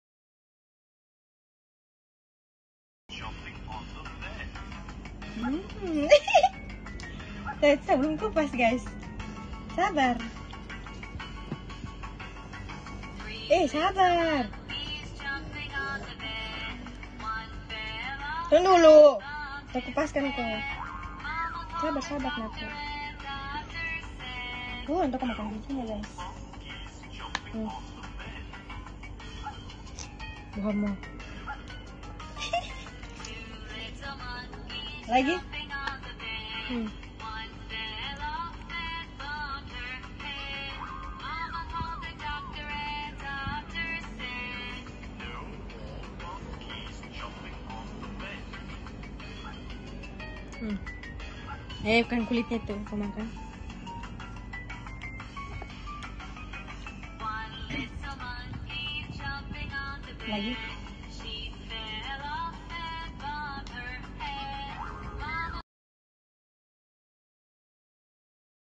Baby Alf ‼️ first time trying to eat oranges
Funny.